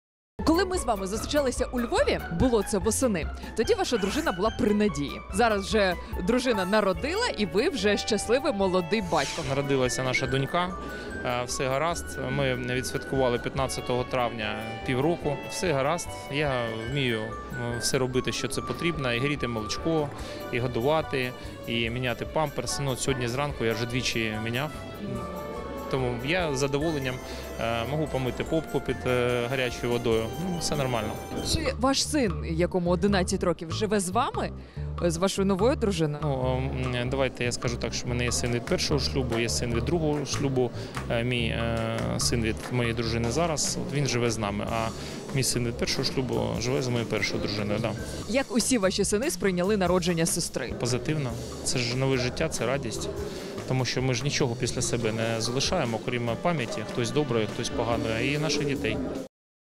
Радник Авакова Антон Геращенко розповів, як міняє памперси маленькій донечці
Нещодавно народний депутат Антон Геращенко став батьком – у сім’ї народилася донечка. Радник міністра внутрішніх справ розповів, як виховує трьох дітей, і з ким живе його син від першого шлюбу.